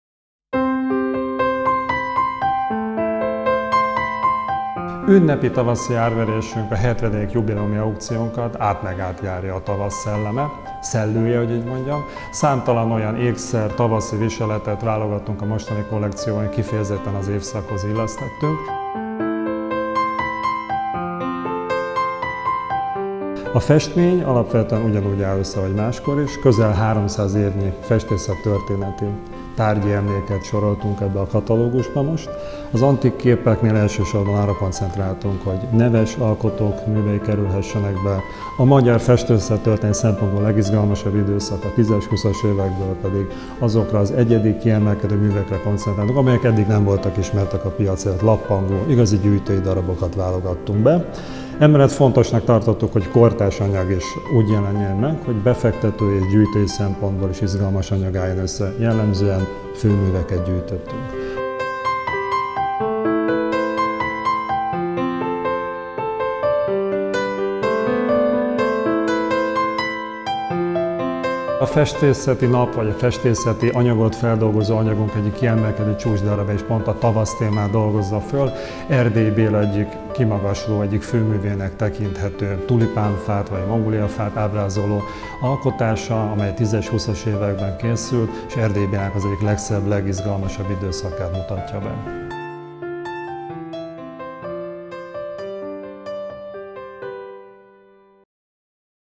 BÁV 70. Művészeti aukció - Festmények
KIÁLLÍTÁS: 2017. május 6–14. (BÁV Aukciósház, 1052 Budapest, Bécsi u. 1.)

ÁRVERÉS: 2017. május 16–17. 17:00
2017. május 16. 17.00 Az ékszerek és ezüst műtárgyak árverése
2017. május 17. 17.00 A festmények és műtárgyak árverése

AZ ÁRVERÉS HELYSZÍNE: MOM Kulturális Központ, 1124 Budapest, Csörsz u. 18.